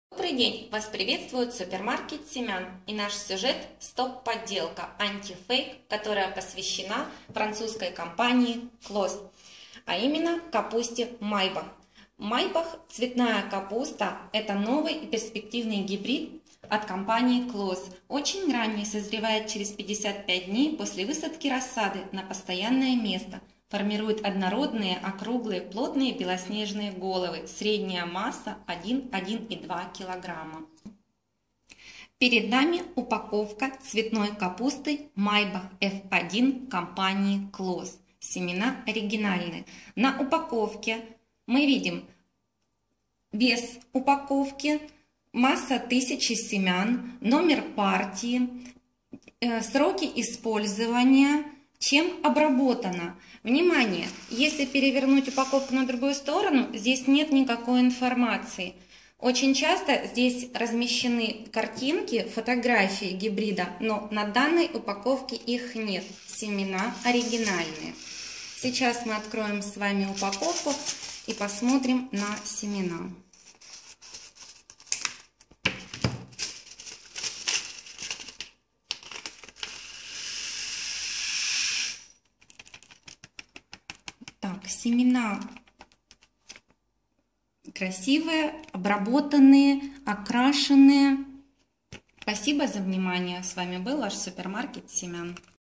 Антифейк. Часть 8. Семена цветной капусты Майбах F1 (Clause)
Вегетационный период - 55 дней. Самый ранний, весенний, для открытого и закрытого грунта. Средний вес 1 кг. Формирует твердые, слегка приплюснутые головки белого цвета.